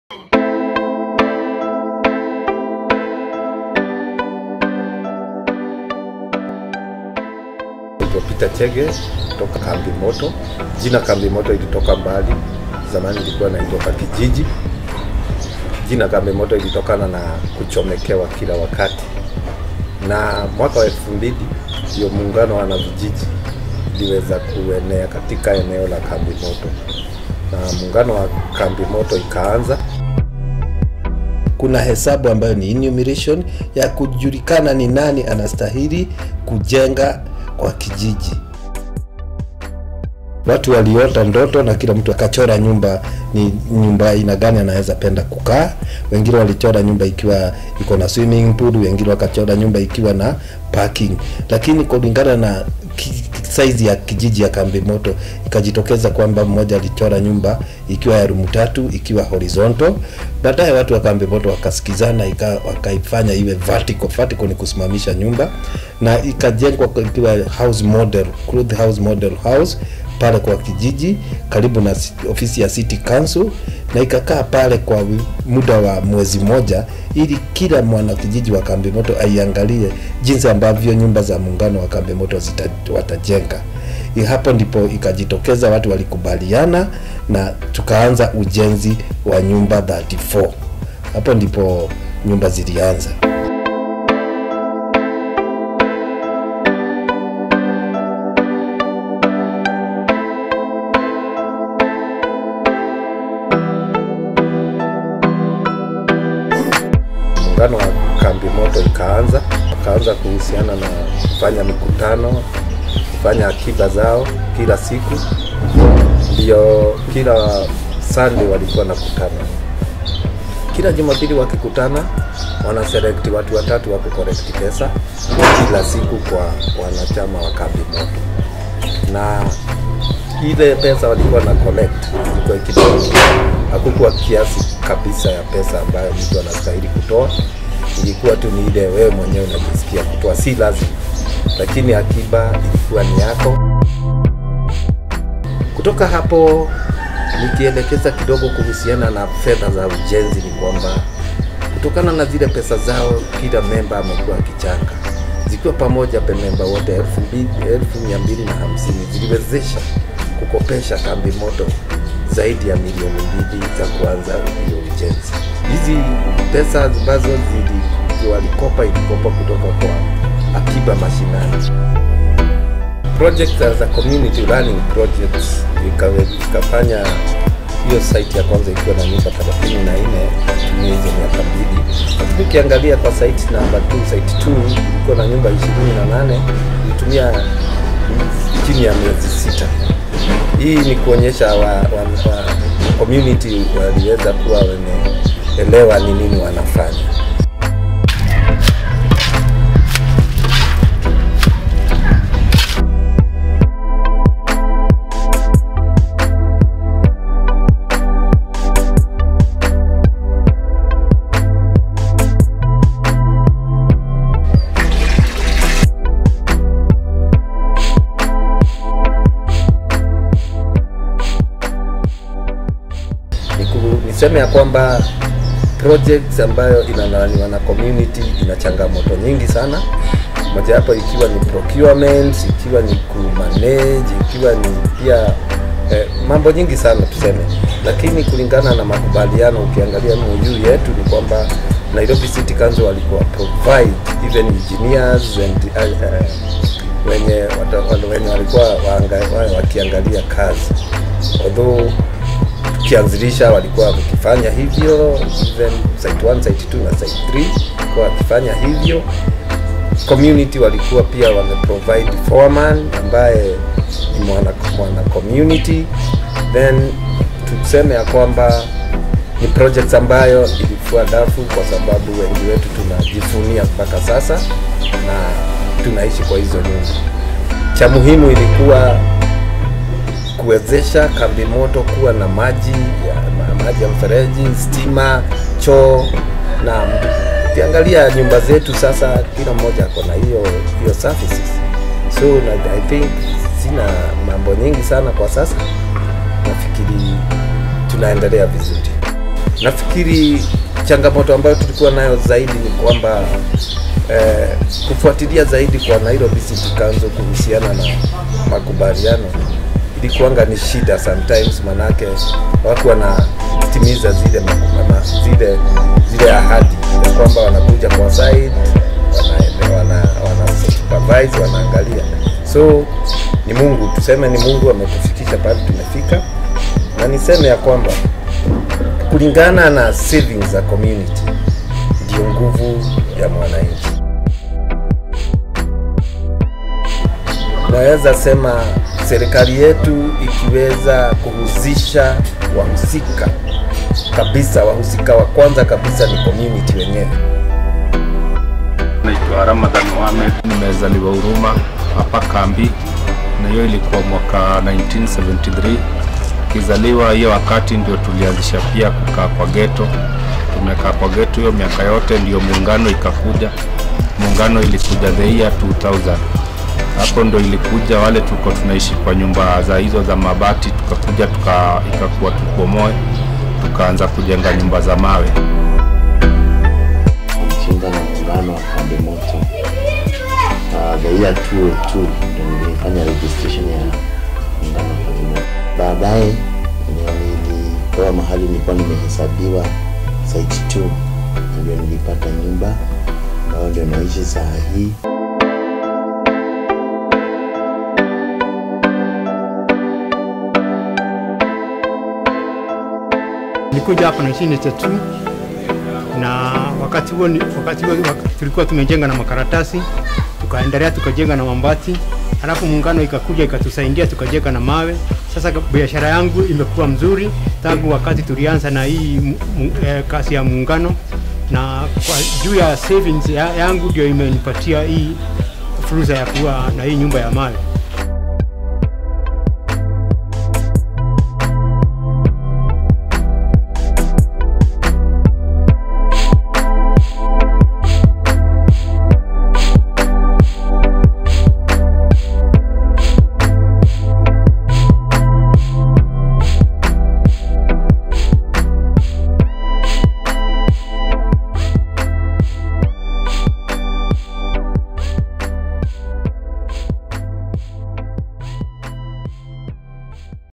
Kambi Moto Housing Project
Filmed by the Kenyan KYCTV team, the short documentary presents one of the federation's housing project in Kambi Moto, where our community was able to develop saving schemes and gain the necessary skills to achieve proper housing for all members.